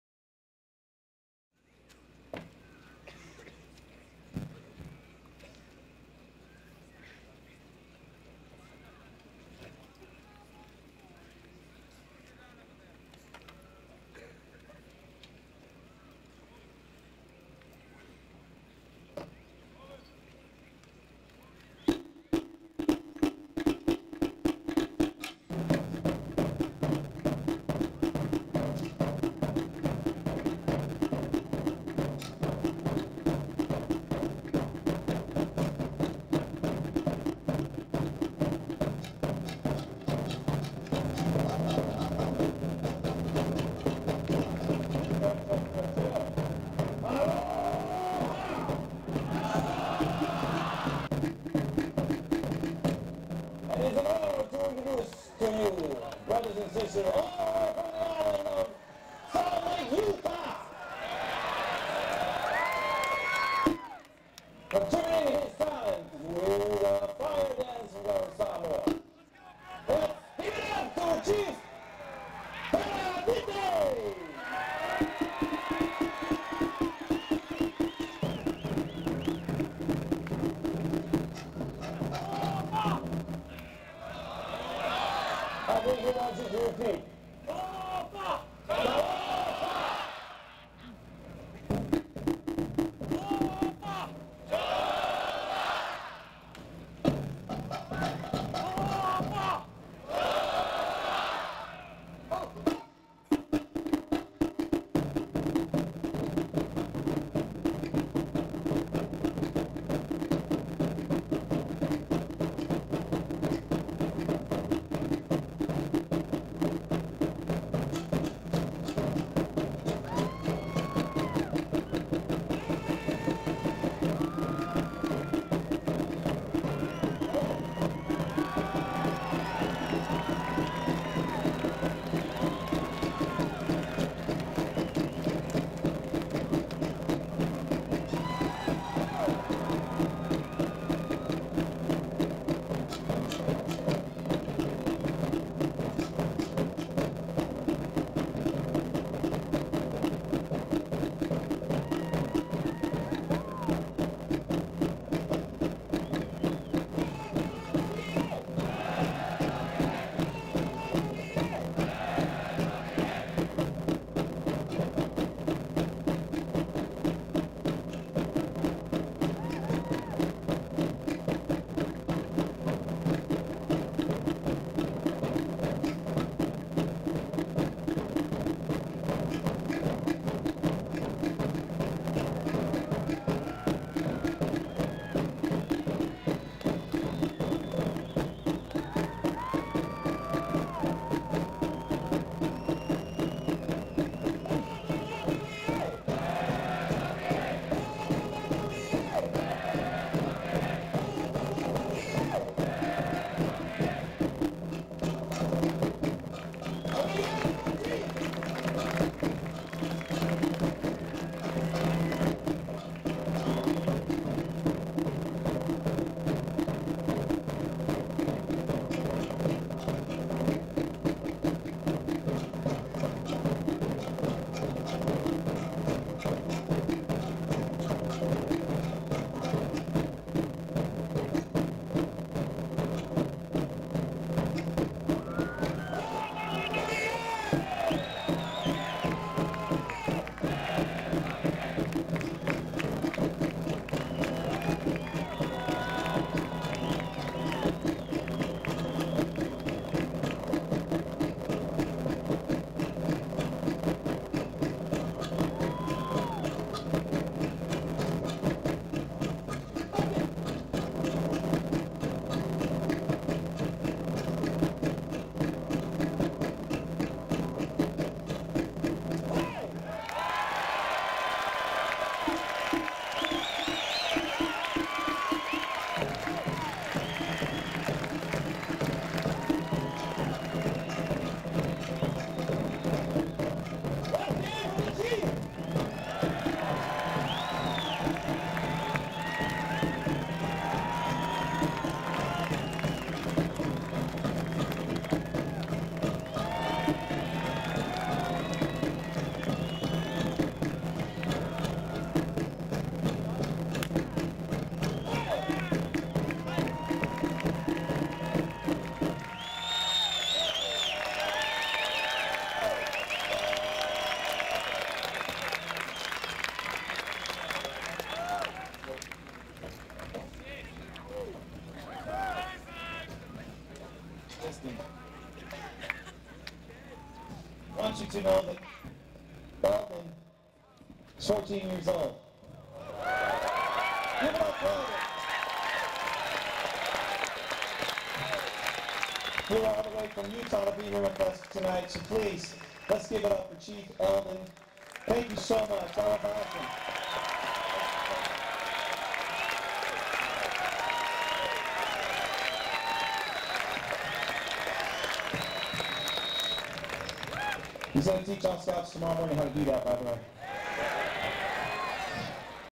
Alton Brown's Fire-knife performance in Sacramento, CA
This was about 2 years ago.  This performance took place in Sacramento, California.  This was at a Scout gathering of about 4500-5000 scouts.